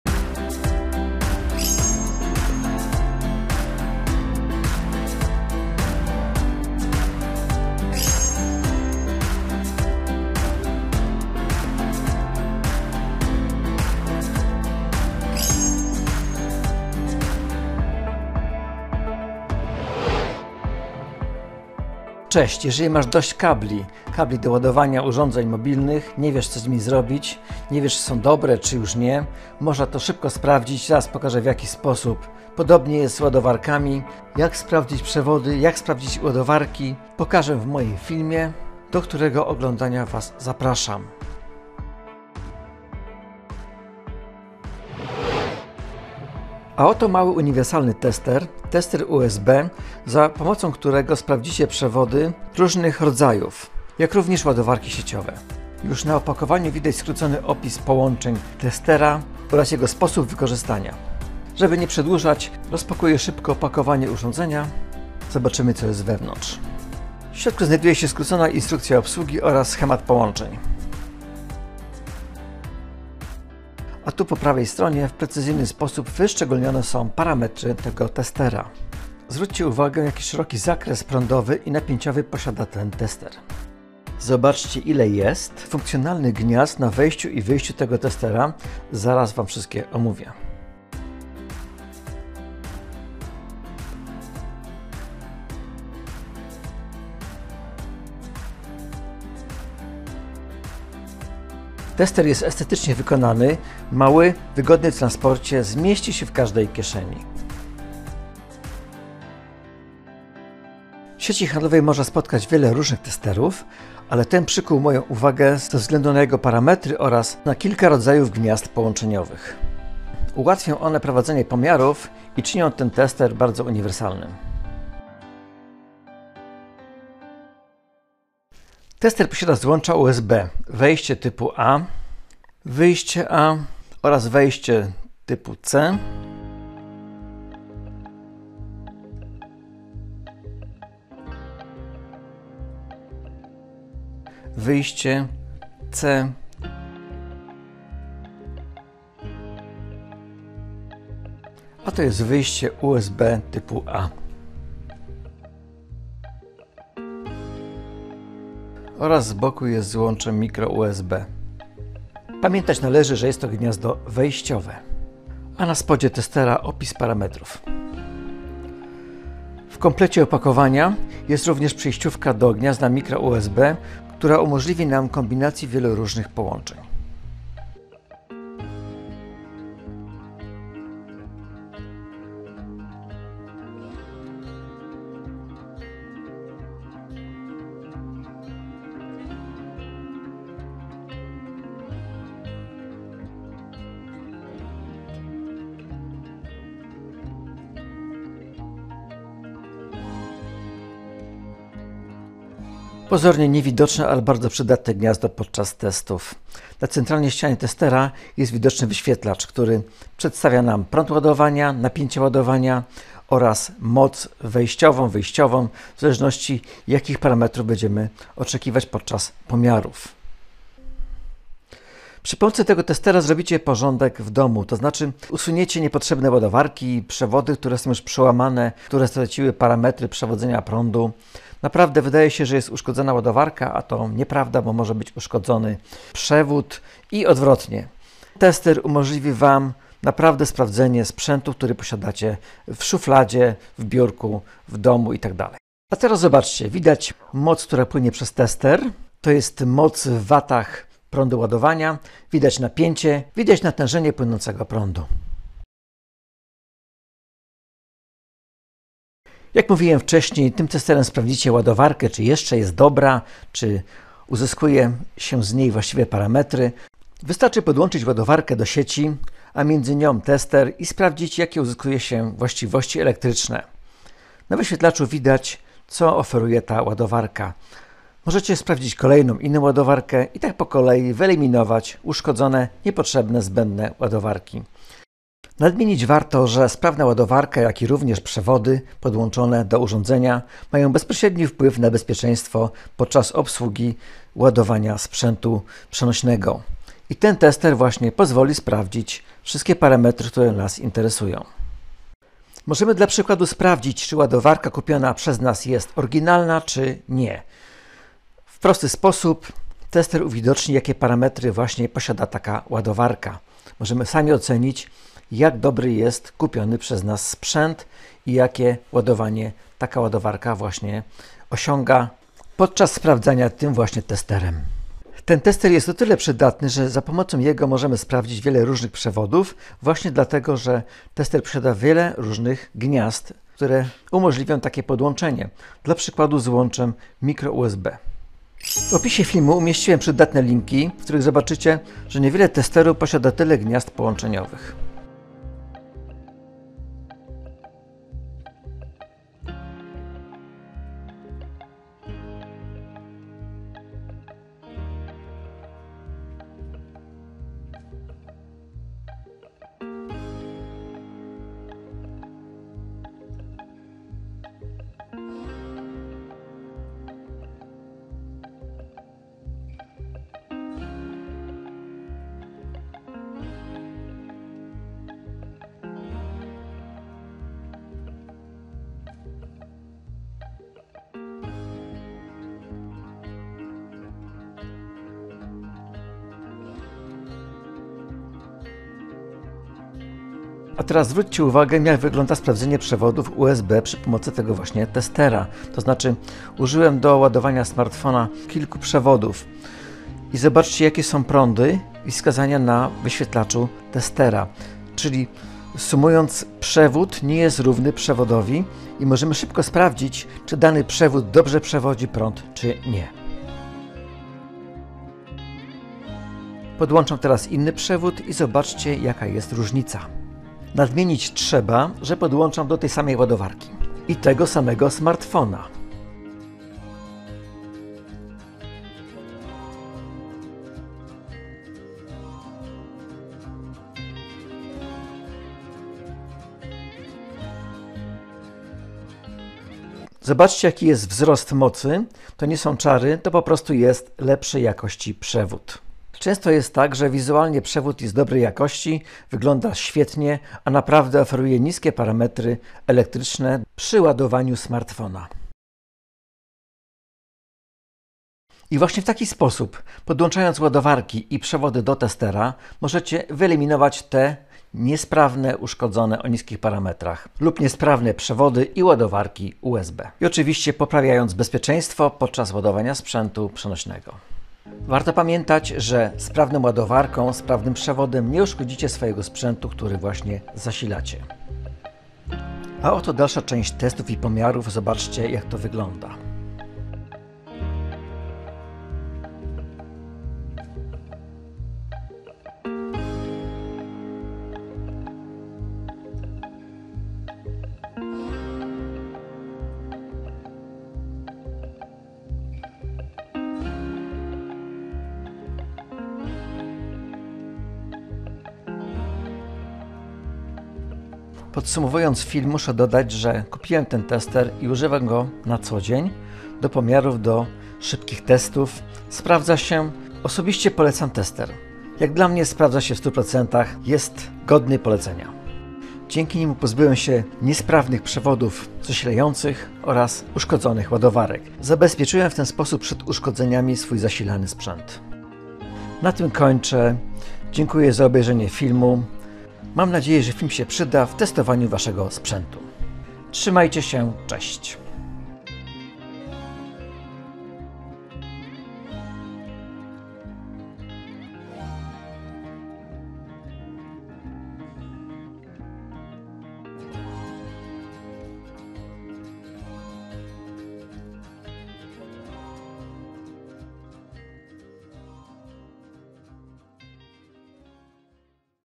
Mały wielofunkcyjny tester USB, tester bezpieczeństwa zasilania.
Witam Wszystkich.
W moim filmie przedstawiam przydatność i sposób zastosowania prezentowanego testera USB.
Za jego pomocą możemy sprawdzić działanie, sprawność ładowarek sieciowych i przewodów zasilających USB.
Tester służy również do sprawdzenia bezpieczeństwa zasilania urządzeń mobilnych.
Zapraszam do oglądania mojego filmu.